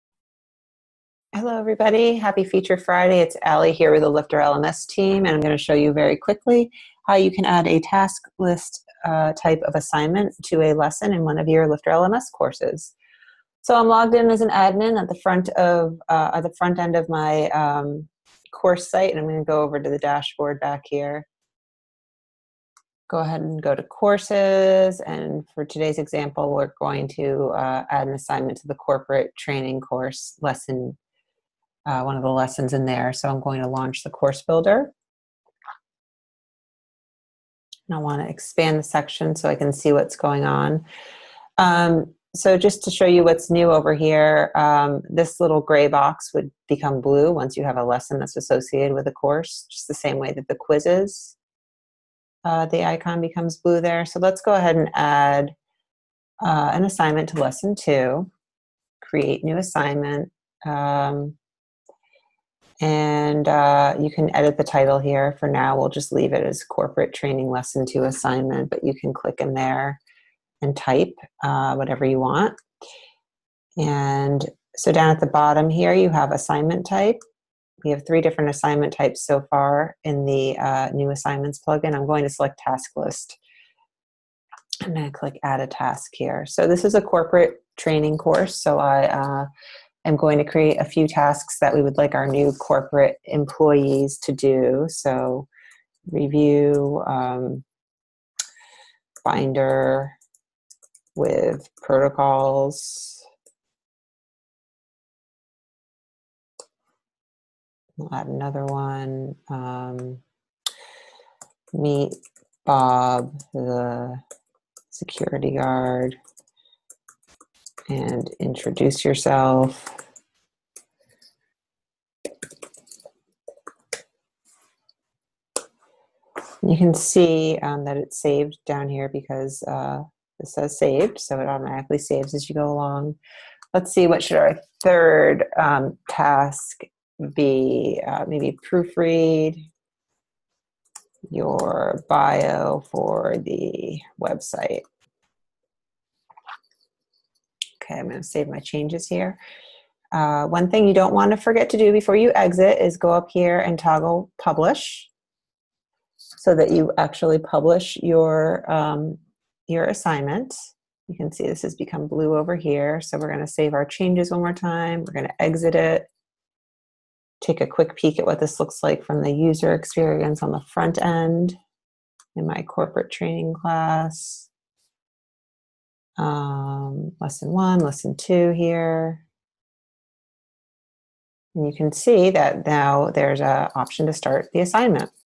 LifterLMS Feature Friday Assignment Task List
In this FeatureFriday video we take a quick look at the Task List Assignment type.